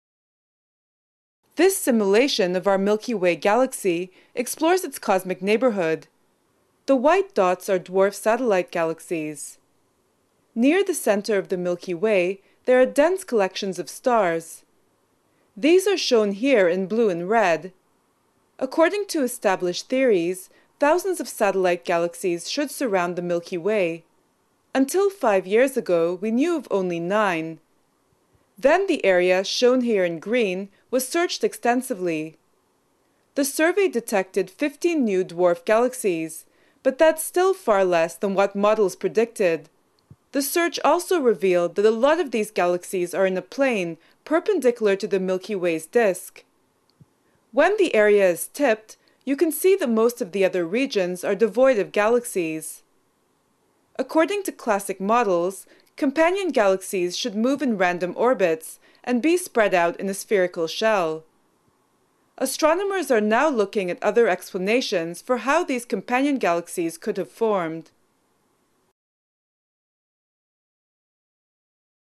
Mystery of the missing mini-galaxies
A simulation has revealed that a galaxy stole stars from its neighbour and could devour it in the future.